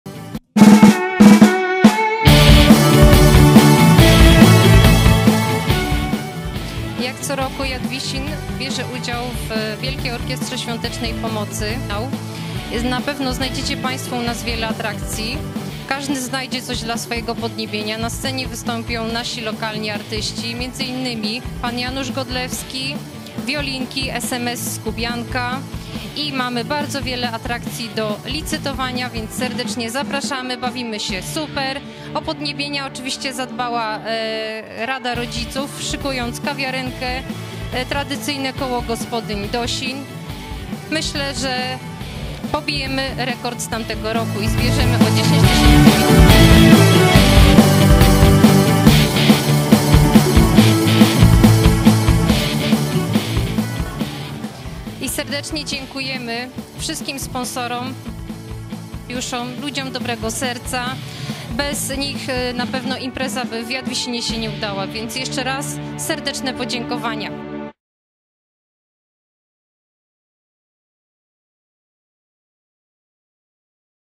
WOŚP 2018. Sztab w Jadwisinie
14 stycznia 2018r., przez cały dzień, 25 wolontariuszy zbierało do puszek pieniądze, które przeznaczone będą na wyrównywanie szans w leczeniu noworodków. Podczas tegorocznego finału jadwisiński sztab zebrał ponad 32 tys. zł. Jednak nie jest to kwota końcowa, gdyż nadal trwają licytacje internetowe – informują redakcję „Gazety Powiatowej” koordynatorzy z tego sztabu WOŚP.